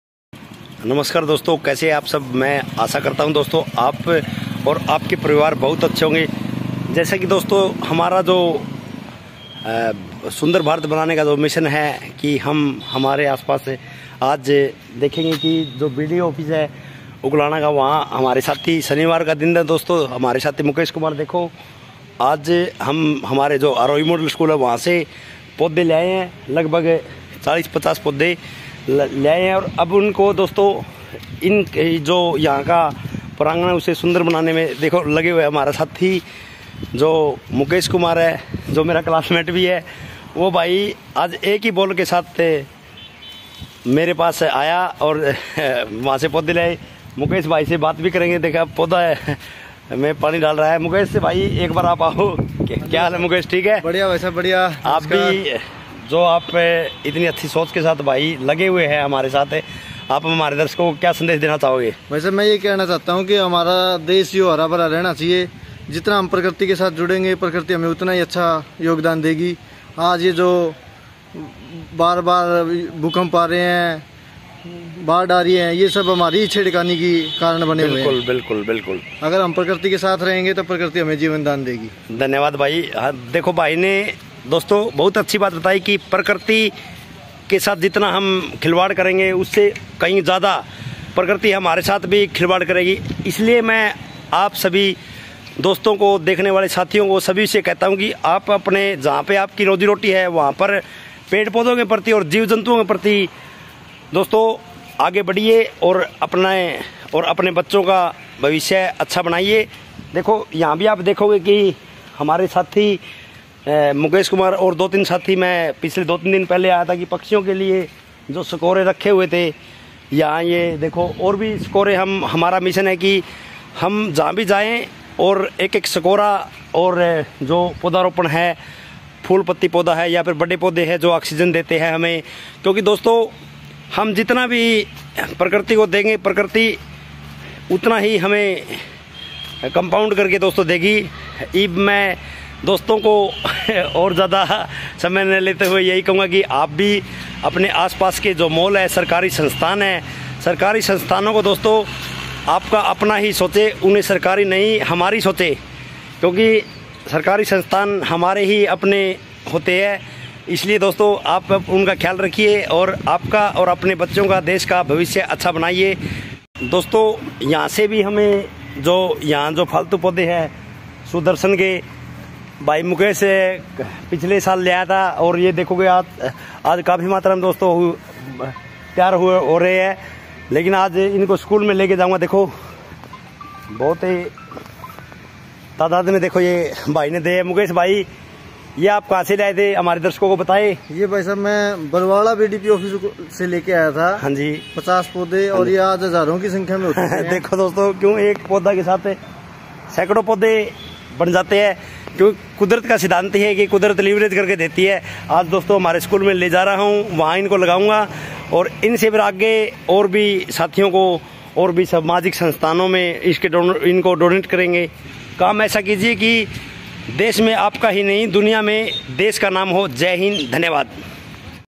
देश को सुंदर बनाने में अपना योगदान दो मेरे साथियों 🙏🏻🙏🏻🌷🌷🌹🌹🌍🌍🙏🏻
नमस्कार दोस्तों जहां  से हमारी रोजी-रोटी चलती है हमारे परिवार का पालन पोषण होता है क्यों ना हम उनको  हमारे घरों को सुंदर बना कर जीव जंतुओं के लिए पेड़ पौधों के लिए एक अच्छी शुरुआत कर कर हमारे बच्चों को अच्छे डायरेक्शन दे जो आप कहते हैं बच्चे वह नहीं करते लेकिन जो आप करते हैं वह देखकर बच्चे जरूर करते हैं समाज के लिए एक अच्छा संदेश जाएगा और आप अच्छे काम की शुरुआत कीजिए हम आपके साथ हैं आप चाहे कहीं से भी हो कमेंट करके आपका नंबर डालिए हम आपसे कॉल करेंगे करो एक अच्छी शुरुआत जय हिंद साथियों🙏🏻